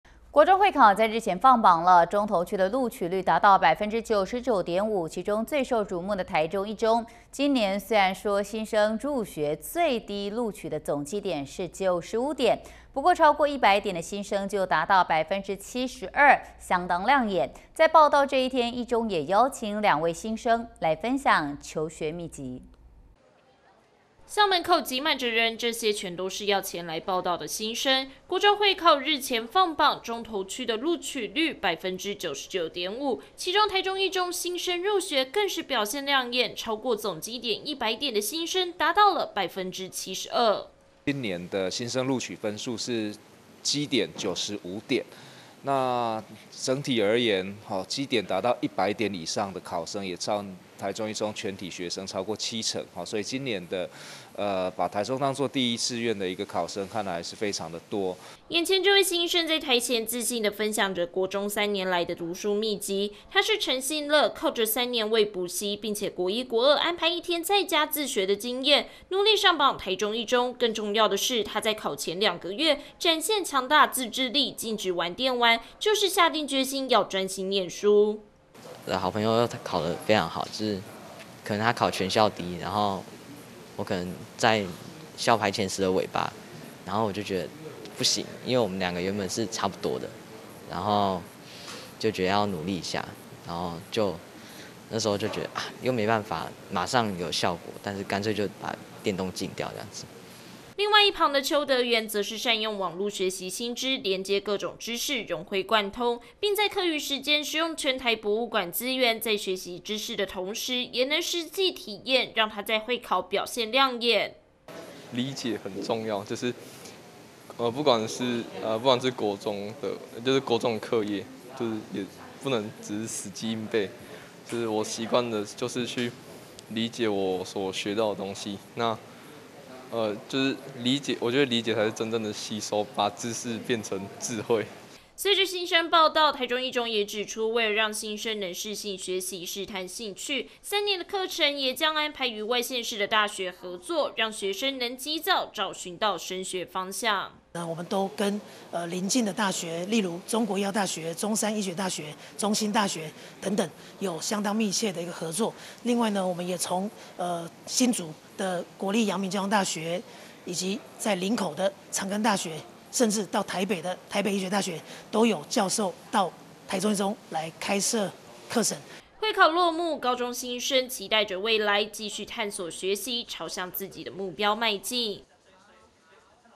國中會考放榜！ 一中新生100積點達72% │T-NEWS聯播網
國中會考在日前放榜，中投區的錄取率達到了99.5%，其中最受矚目的台中一中，今年雖說新生入學最低錄取總積點為95點，不過超過100點的新生，就達到了72%，相當亮眼，在報到這天，一中也邀請2位新生分享求學秘笈。